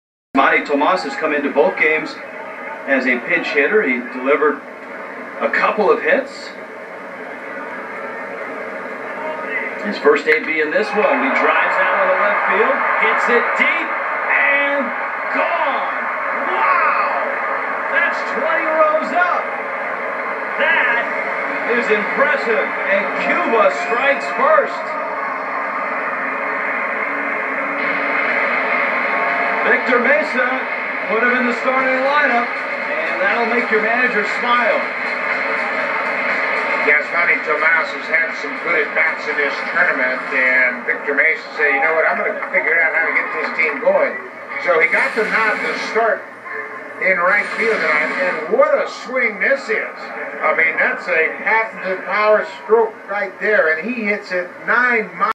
Yasmani Tomas hits a monster home run for Cuba in World Baseball Classic 2013
Cuban baseball player Yasmani Tomas his a big home run in a 6-3 win over Japan in the World Baseball Classic in Fukuoka, Japan, on March 6, 2013. He hits the ball about 20 rows into the stands in left field off Japanese star pitcher Kenji Otanari.

El jugador de béisbol cubano Yasmani Tomás realiza un casa grande correr en la victoria por 6-3 sobre Japón en el Clásico Mundial de Béisbol en Fukuoka, Japón, el 6 de marzo de 2013. Golpea la pelota alrededor de 20 filas en las gradas en el jardín izquierdo frente a la estrella japonesa lanzador Otanari Kenji. Este video fue tomado de la televisión estadounidense. Creo que Tomás podía jugar en las Grandes Ligas de Béisbol en los Estados Unidos. Japón es un buen equipo, pero Cuba es grande.

キューバの野球選手Yasmaniトマスは2013年3月6日に福岡、日本、ワールドベースボールクラシック（WBC）で日本全国の勝利6-3で実行する大きな家を打つ。彼は日本のスター投手健二Otanariオフ左側のフィールドに立っているに20行程度ボールを打つ。この動画は、アメリカのテレビから撮ったものです。私は、トマスはアメリカでメジャーリーグで野球をすることができると思います。日本は良いチームだが、キューバは素晴らしいです。

쿠바 야구 선수 Yasmani 토마스는 2013년 3월 6일에 후쿠오카, 일본에서 월드 베이스볼 클래식에서 일본에 비해 6-3 승리에서 실행 큰 집에 도달. 그는 일본의 스타 투수 켄지 Otanari에서 왼쪽 필드에서 스탠드에 20 행에 대해 공을 공격합니다. 이 동영상은 미국 TV에서 촬영되었다. 나는 할아버지가 미국에서 메이저 리그에서 야구를 재생할 수있을거야. 일본은 좋은 팀이지만, 쿠바는 정말 멋진 여자에요.

古巴棒球选手Yasmani托马斯命中以6-3战胜了日本在世界棒球经典赛2013年3月6日在日本福冈市，运行在一个大的家庭。他打的球约20行到关健二Otanari日本明星投手在左外野看台上。这部影片是从美国的电视。我认为托马斯可以发挥在美国职业棒球大联盟。日本是一个很好的球队，但古巴是伟大的。